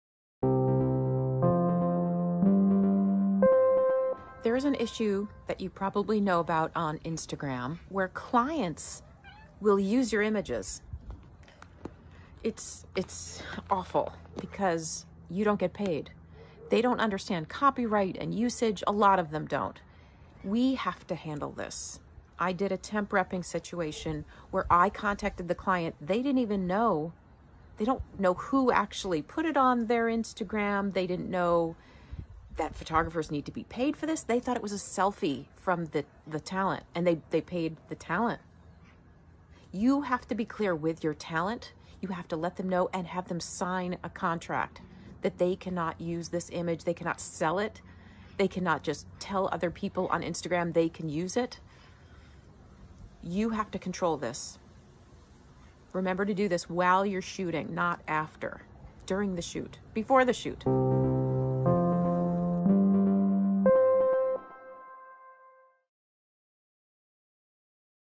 AskSternRep on Instagram Permission
Commercial Photographer's Rep Andrea Stern of SternRep + AskSternRep discusses communication with crew, clients, and talent that you own the images.